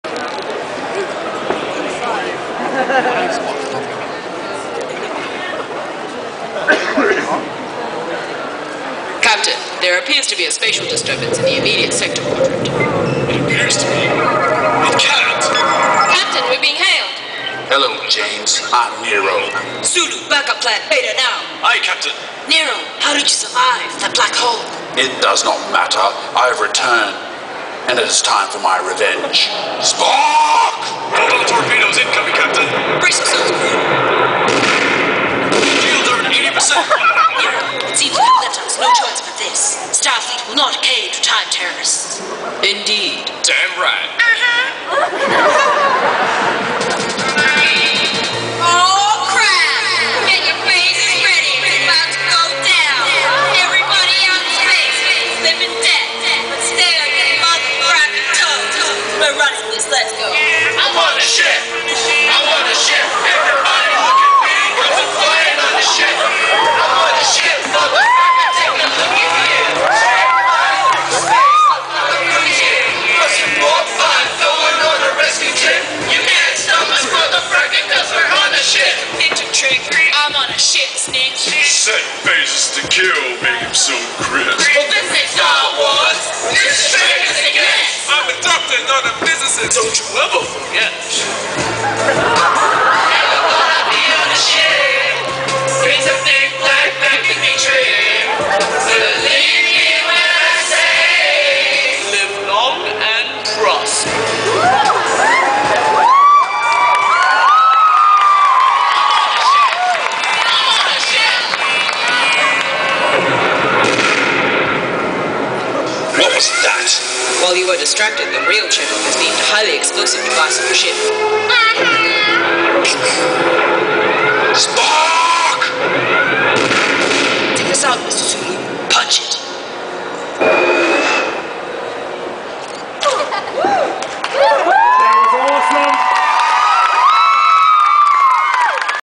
Supanova Star Trek Skit 2010
STARTREK!STARTREK!STARTREK!STARTREK!STARTREK!

OMFG. YOU GAIZ KILLED ME SO MANY TIMES OVER WITH THIS SKIT. I JUST HAD TO POST THIS UP HAHAHAHAHAH I FRIKKEN LOVE YOU ALL!!!

Cosplayers: Steve, Alita, Ailee, Suri, and Anna! ^__^
Convention: Sydney Supanova 2010

LMAOO WE'RE ON A SHIP. OMFG. WINNNNNNNNNNNNN. But garghhh sorry for the shitty quality & angle TAT;;;; OTL OTL OTL